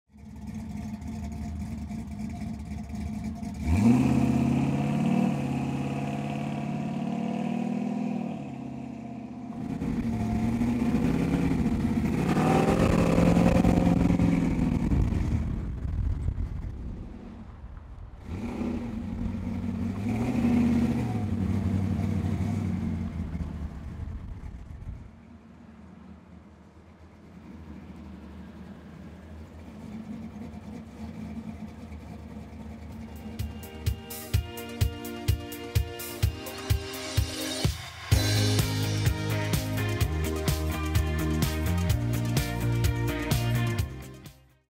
1966 Chevrolet Nova - Stock 137830
Pro-Touring nova Custom 6.0L LS2 V8 automatic Swartz Chassis Leather A/C
Pro-Touring Nova custom
Torch red exterior with custom striping
All aluminum 6.0L LS2 crate engine
Electronic Fuel Injection
Custom Swartz full tube chassis incorporated into unit body
4L60E Automatic Transmission
Power rack and pinion steering
Wilwood 13" Power Disc Brakes front and rear
Air Conditioning
Dual 3" Flowmaster exhaust
18" chrome ZE Forged brand wheels with BFGoodrich G-Force tires
Full custom tan bucket seat interior
Show quality engine compartment
Upholstered trunk compartment
Coil over shocks front and rear
All RK Motors vehicles receive a comprehensive, multi-point inspection
We can ship your new purchase anywhere in the world